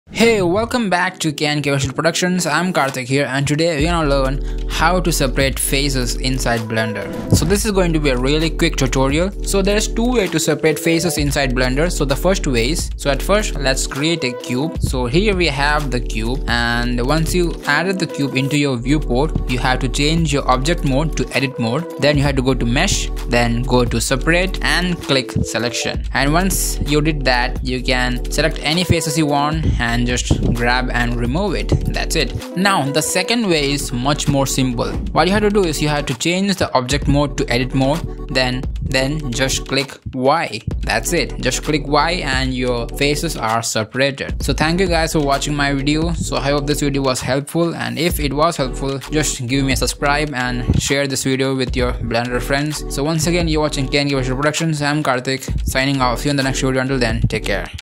Blender 3.4 Tutorial: Separate A Face Of An Object.
Welcome to my Blender tutorial on face separation! In this video, I'll guide you through the process of separating faces in Blender, allowing you to enhance your 3D modeling and animation skills.